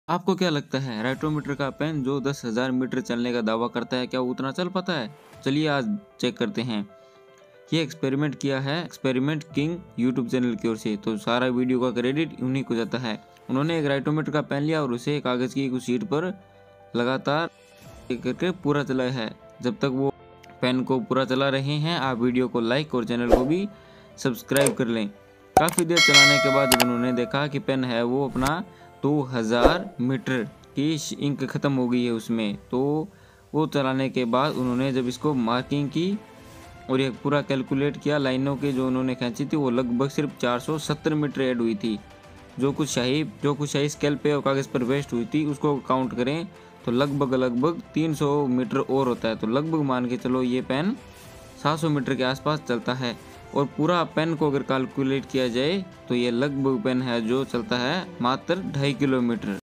writo–meter का pen कितना चलेगा । Jangir Classes
video credit – Experiment king